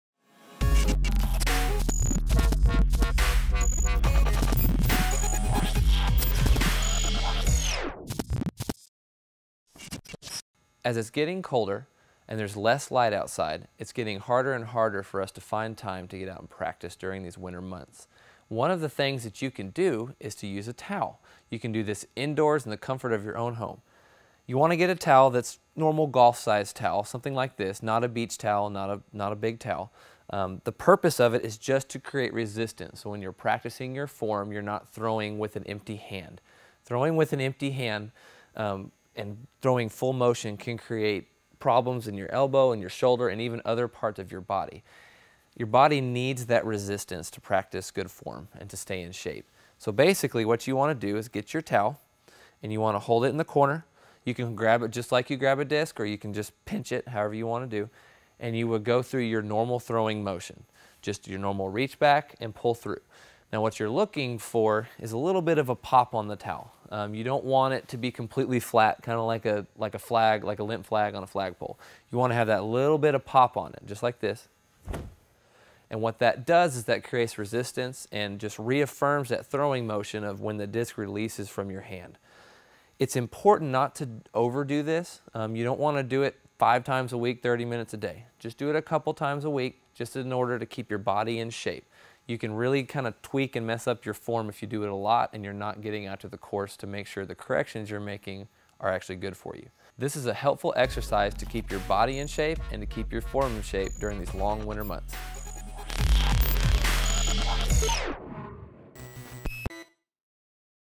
Practice with a Towel | Disc Golf Tip
For most areas of the world winter time means colder temperatures and less sunlight which translate to less time on the disc golf course. Even though you are unable to go outside and throw some plastic you can still use this tip to keep your form in check and ready for the next disc golf season.